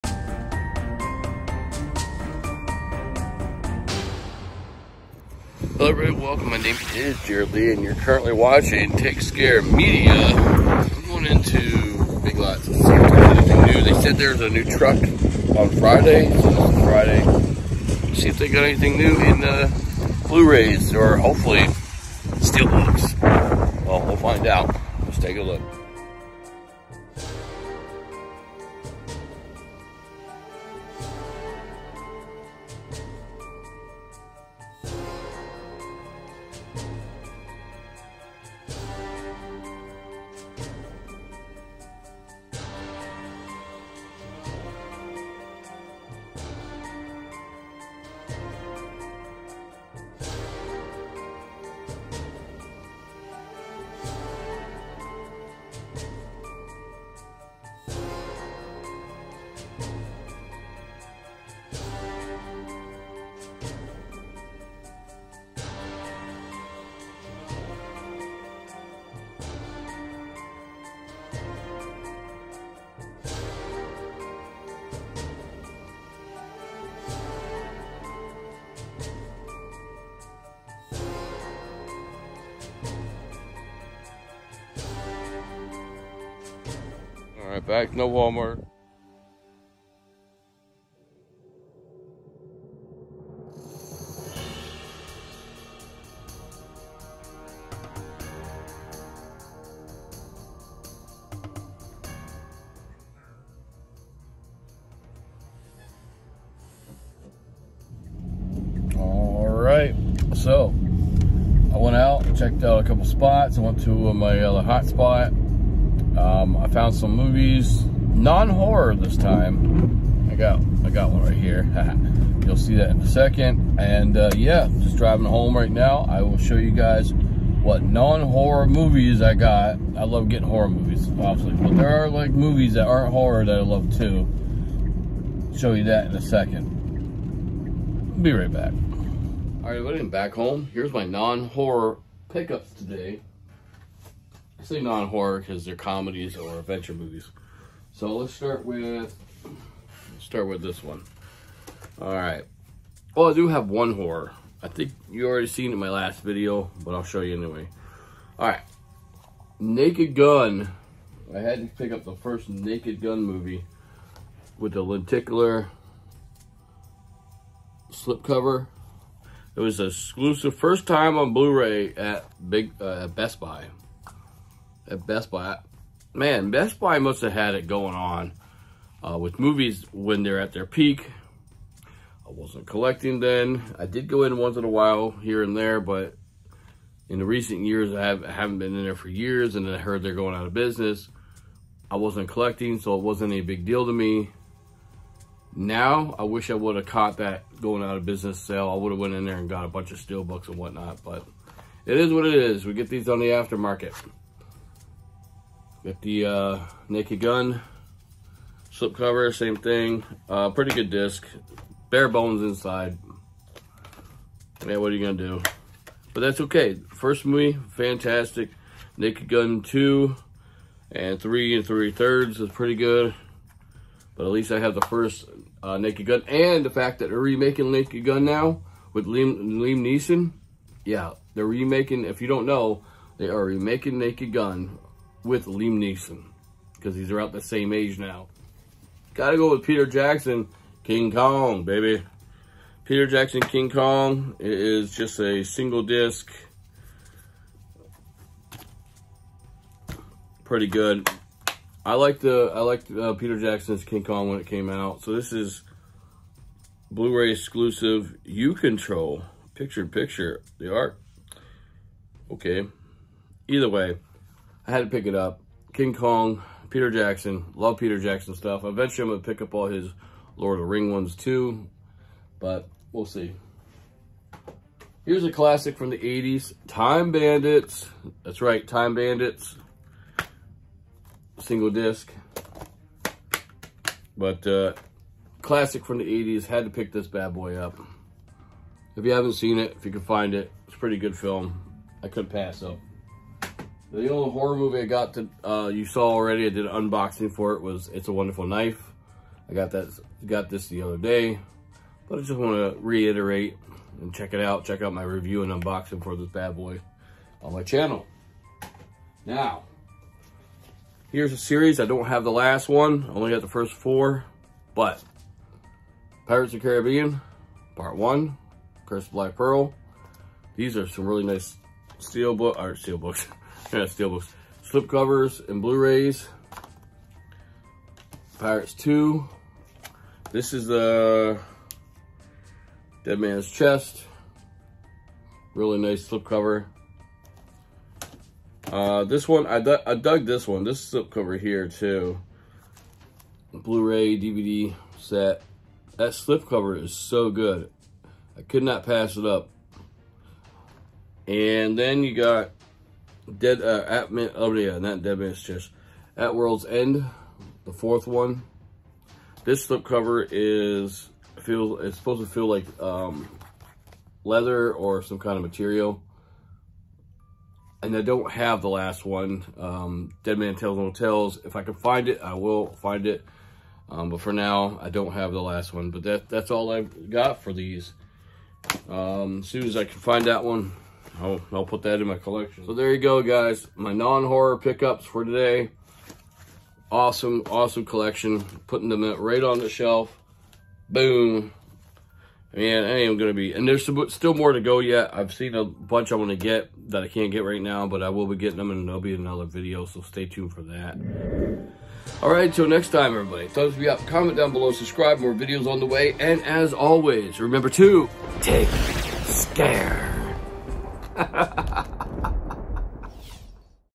HUNTING: NON HORROR BLU-RAY Pick Ups /
On today’s TSM: I head out in search of Blu-Rays .